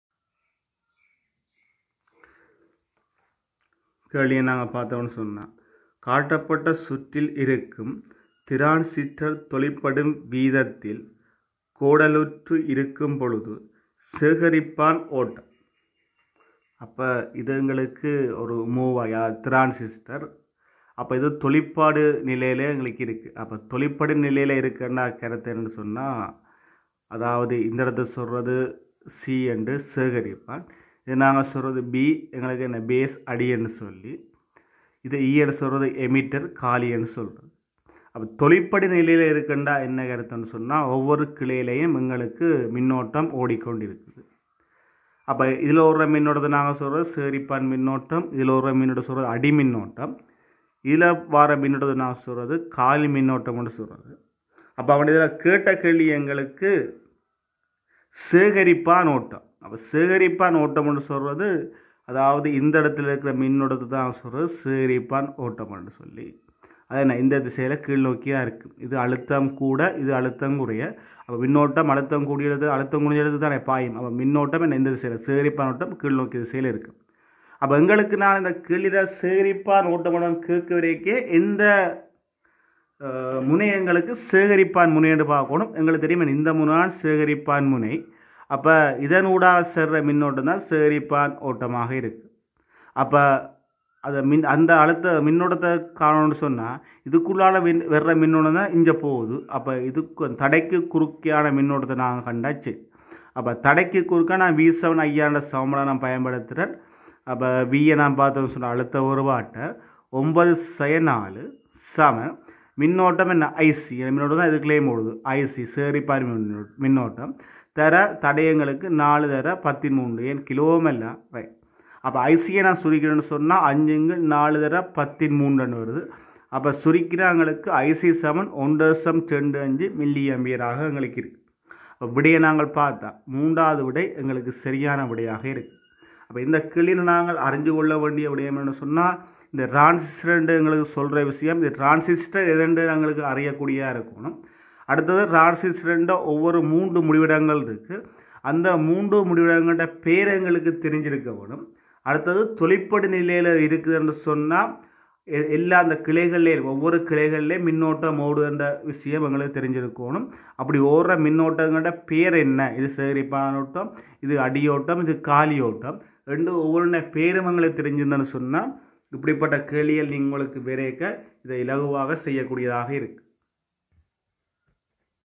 2017 physics MCQ/ 18 - Tamil medium
The first in a series of videos initiated by StudyBench. Here, MCQ questions 18 from the physics paper of the year 2017, are discussed.

கடந்த கால வினாக்களிற்கான விளக்க காணொளி
2017 – பௌதீகவியல்

உயர்தர கணித விஞ்ஞான பிரிவு மாணவர்களிற்கான பௌதீகவியல் பாடத்திற்கான 2017 ம் ஆண்டு கடந்தகால வினா தொடர்பான குறிப்புக்கள்